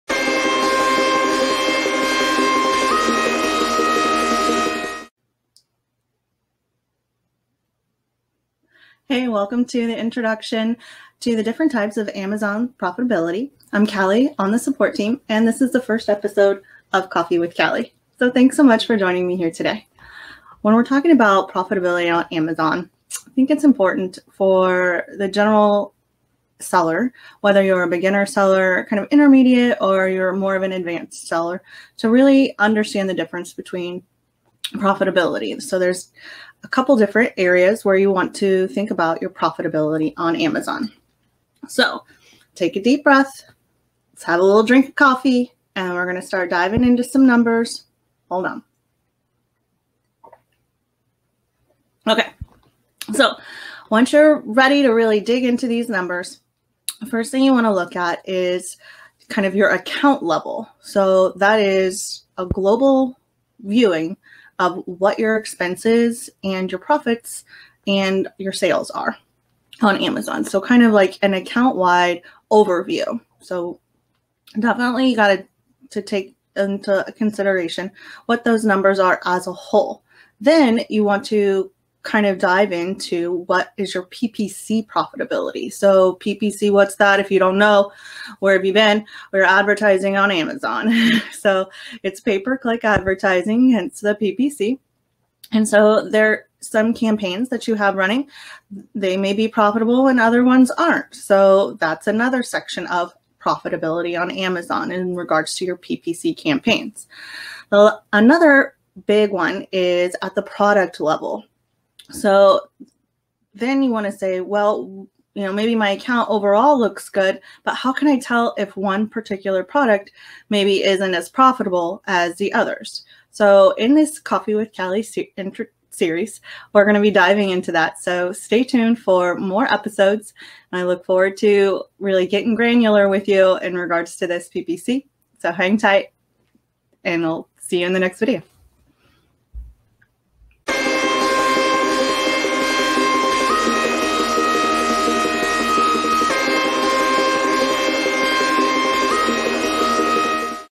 An Introduction To The Different Types Of Amazon Profitability
Metrics to consider when analyzing the Profitability of your Amazon selling business with Kali on the Support Team.

What part of this video was the most helpful?
Sellozo has an Amazing set of tools to help you automate your Amazon advertising.